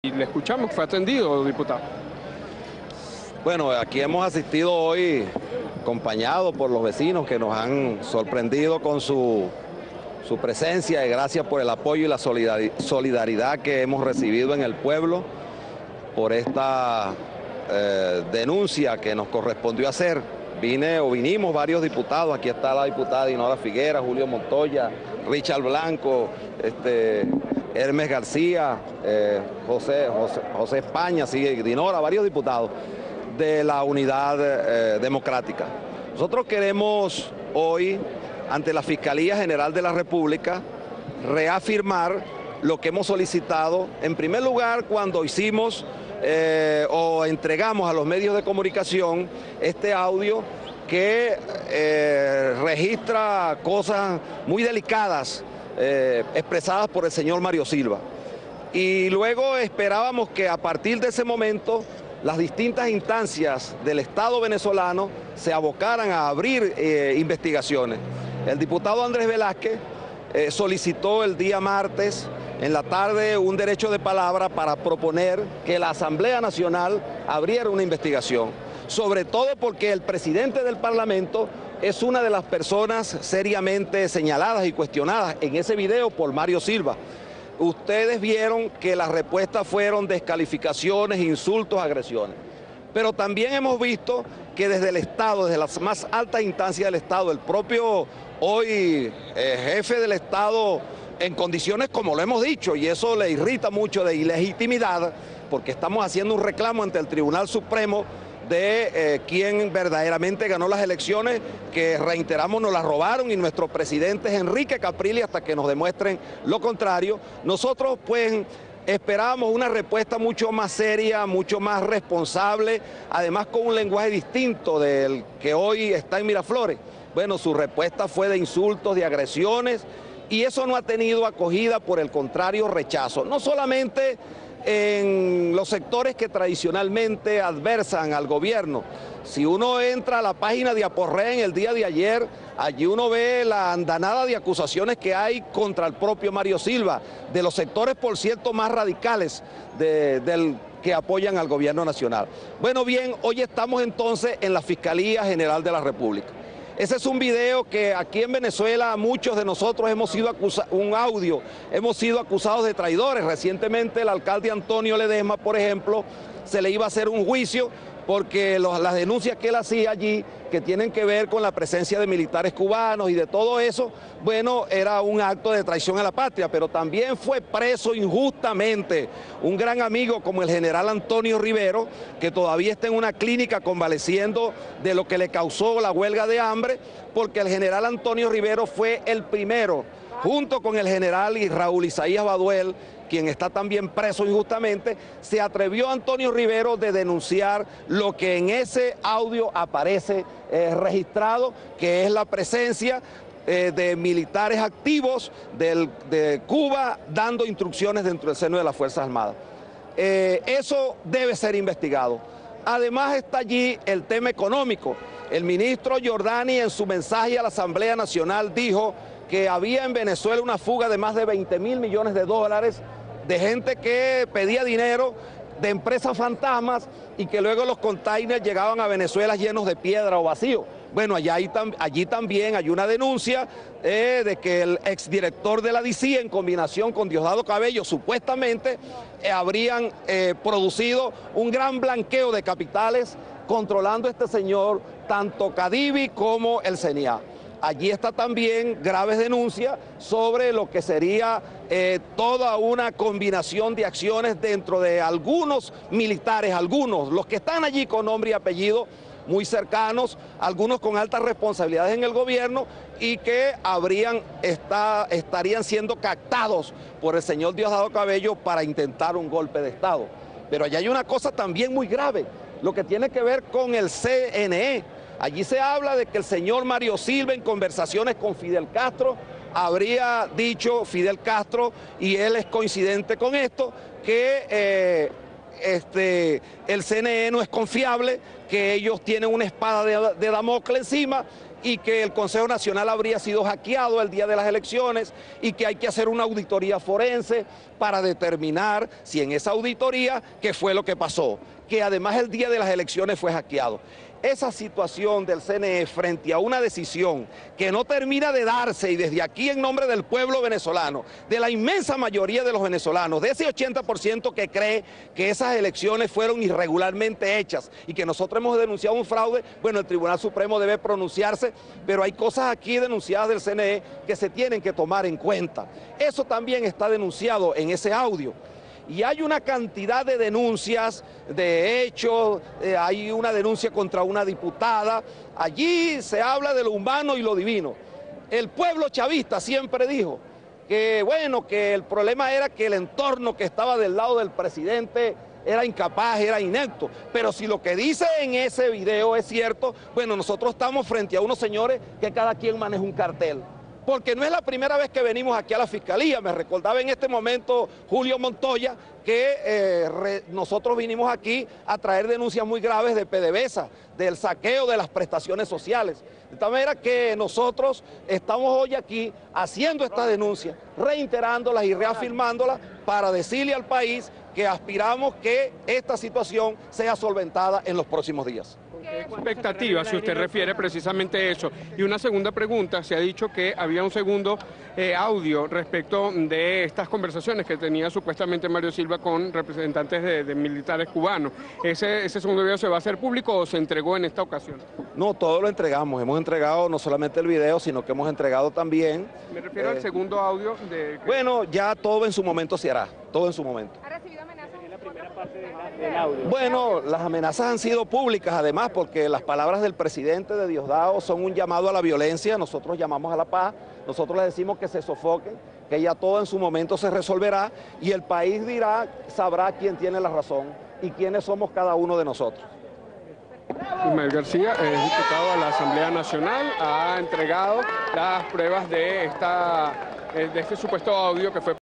Ismael García sobre un segundo audio: "Todo en su momento se hará"
El diputado Ismael García señaló que "todo en su momento se hará", al ser indagado sobre un segundo audio que la MUD dará a conocer al país